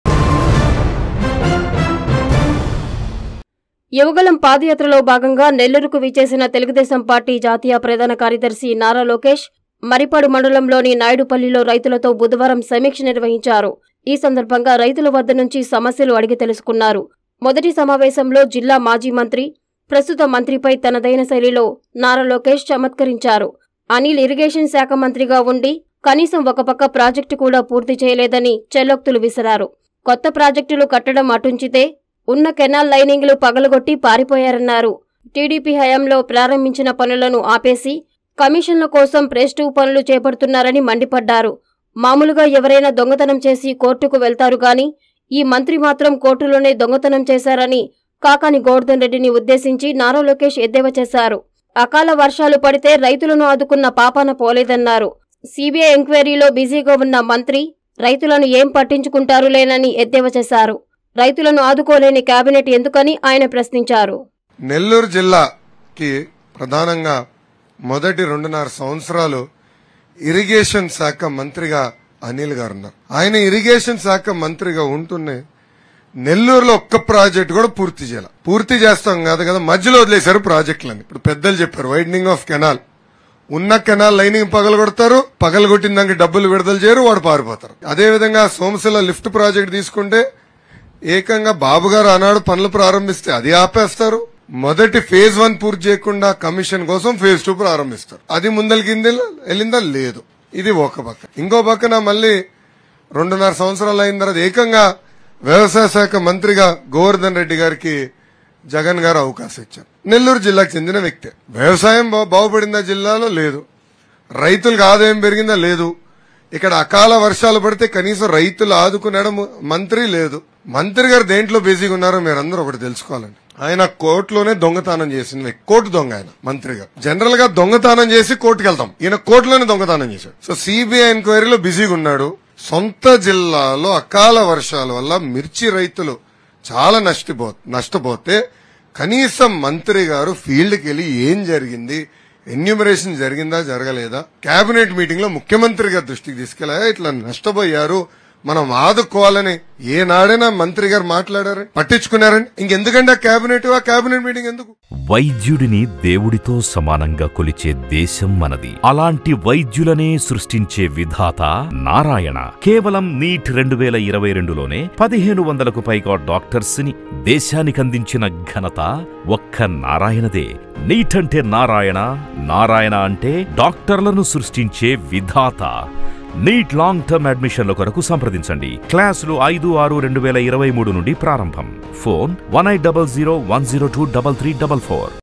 కోర్టుదొంగ... మంత్రి కాకాణి..! | Lokesh TDP
మంత్రిగా అనీల్ క‌ట్టిన ప్రాజెక్టులు ఏవీ..?
నాయుడుప‌ల్లె వ‌ద్ద జ‌రిగిన రైతుల స‌మీక్ష‌లో ప్ర‌శ్నించిన నారా లోకేష్‌